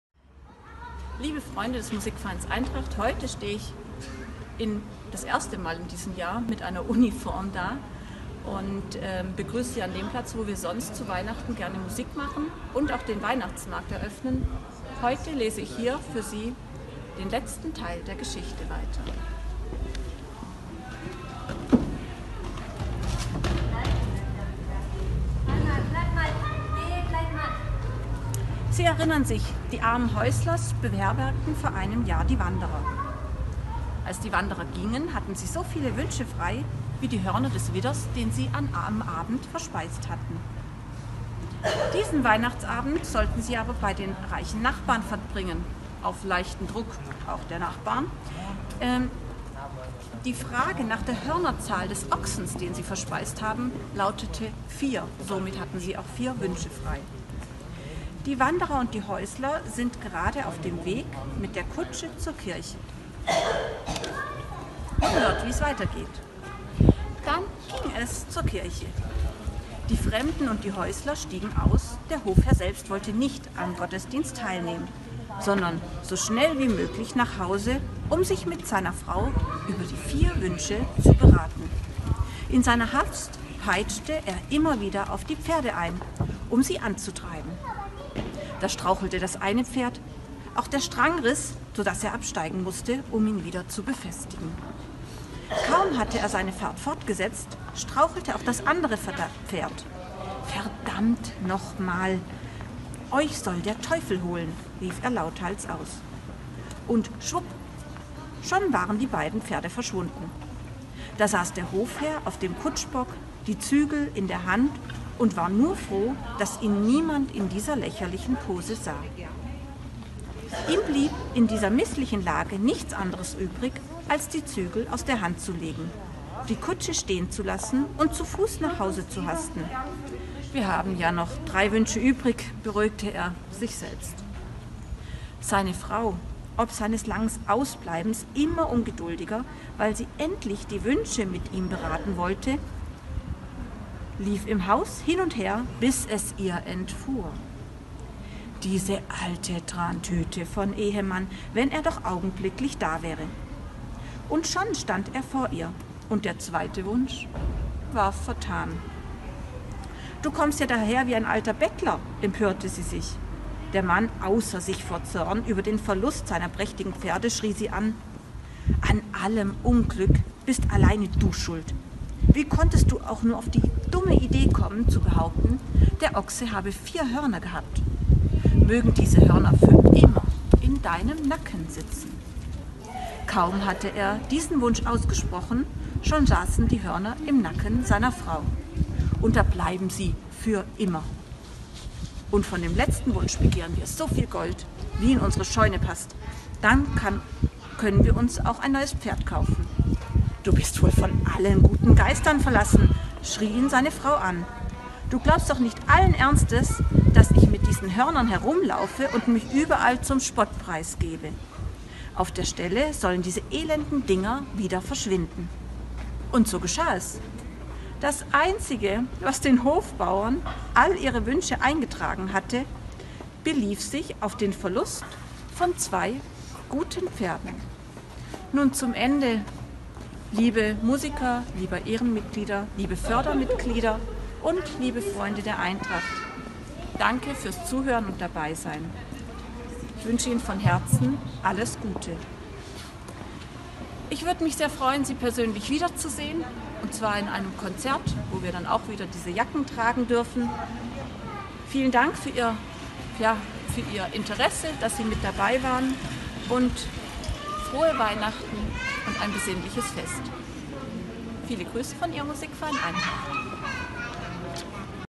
Adventskalender 2020 [23] Musikverein Eintracht Petershausen Konstanz
Die Weihnachtsgeschichte von Monika, Teil 4.

Da wir im Corona Jahr 2020 leider kein einziges Konzert geben konnten, haben wir für die Weihnachtszeit einen Adventskalender eingerichtet. 24 spannende Türchen mit Beiträgen von unseren Musikern und Schülern.